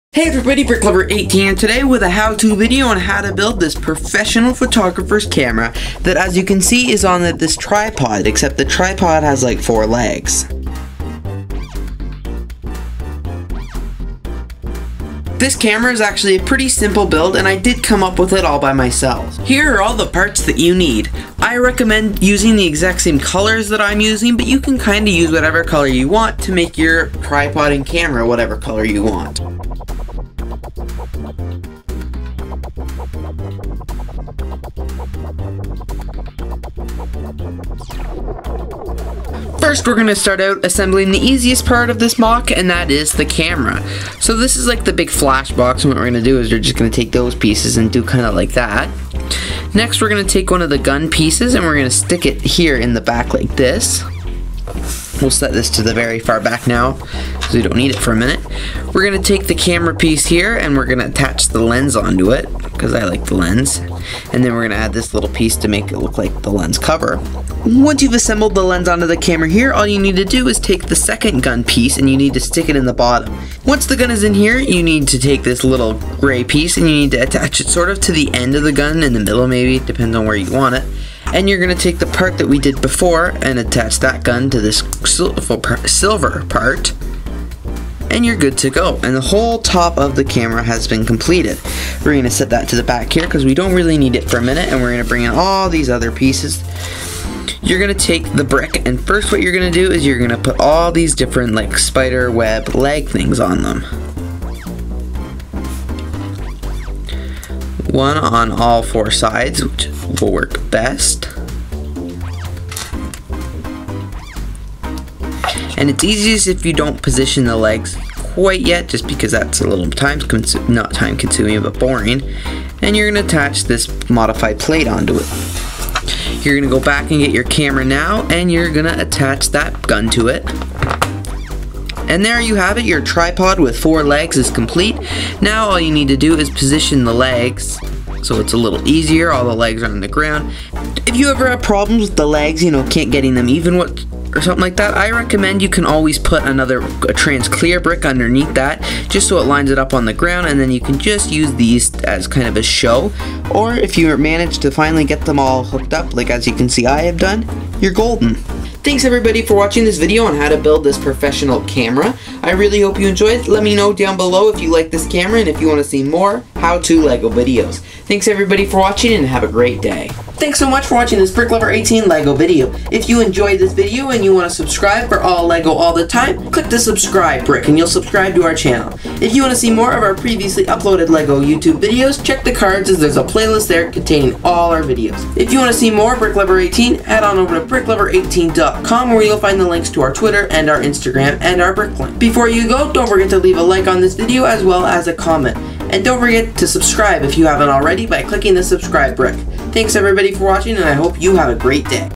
HOW TO BUILD A LEGO CAMERA
If your mini figures need a professional looking LEGO photographers camera, this is the video you need!